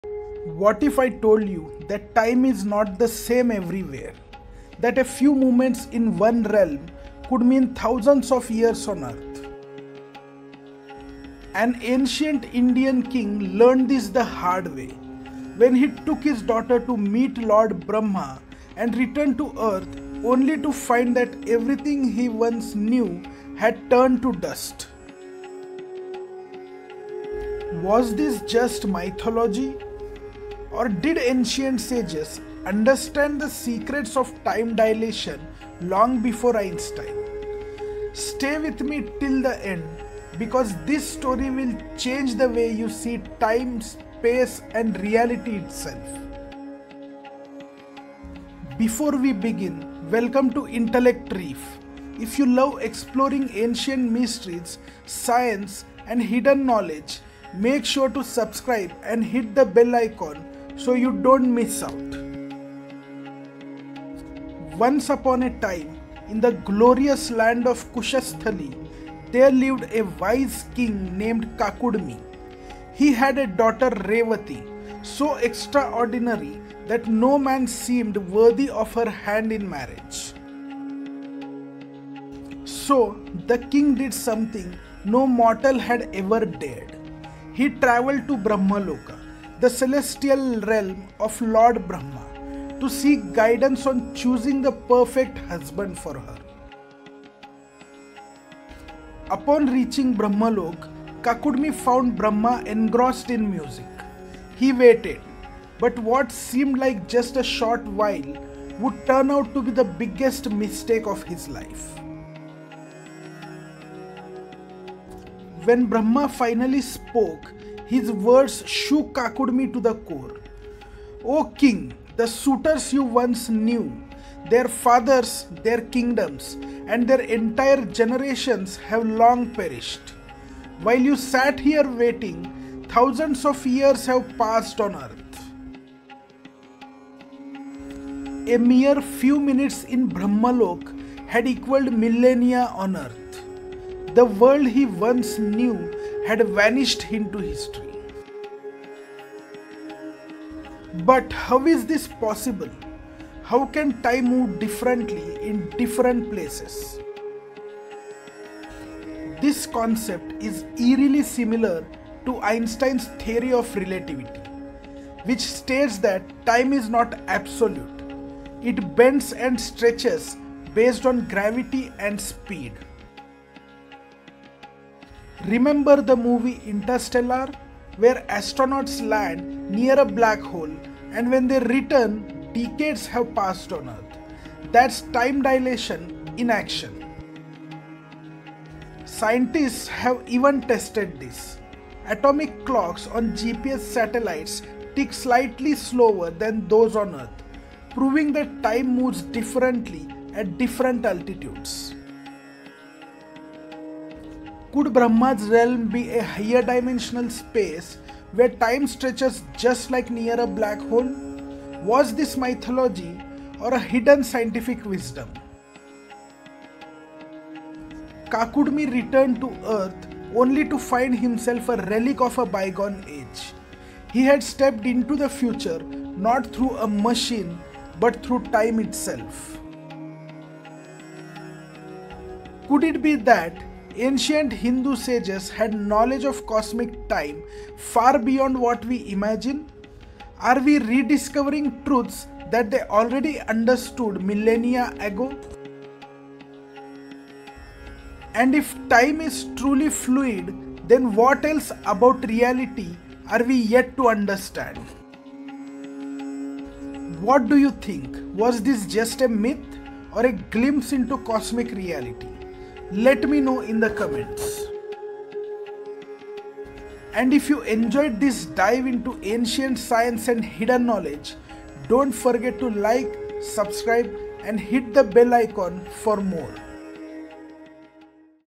Kakudmi & The Secret of Time Dilation | Did Ancient Sages Know Relativity?
Kakudmi & The Secret of Time Dilation | Did Ancient Sages Know Time Dilation?

What if time is not what we think it is? What if a few minutes in one realm could mean thousands of years on Earth? 🤯
This is the mind-blowing story of King Kakudmi from ancient Hindu scriptures—a king who traveled to Brahmaloka (the celestial realm of Lord Brahma) for what seemed like a short visit… only to return and find that millennia had passed on Earth.
But is this just a mythological tale, or does it hide scientific truth? Einstein’s Theory of Relativity tells us that time is not absolute—it bends and stretches based on gravity and speed. Was this knowledge already embedded in ancient texts long before modern physics discovered it?
Join me in this cinematic deep dive into Hindu cosmology, time dilation, relativity, and the mysteries of space-time itself. 🚀

🔍 Topics Covered in This Video:
 ✅ The story of King Kakudmi and Revati in Hindu mythology
 ✅ Time Dilation and Einstein’s Theory of Relativity
 ✅ Interstellar’s black hole time-warp vs. Brahmaloka’s time shift
 ✅ Ancient Hindu cosmology & the concept of time
 ✅ Is Hindu mythology actually ancient science?

🎵 Credits
 🎨 AI-Generated Images: Created using AI tools for storytelling enhancement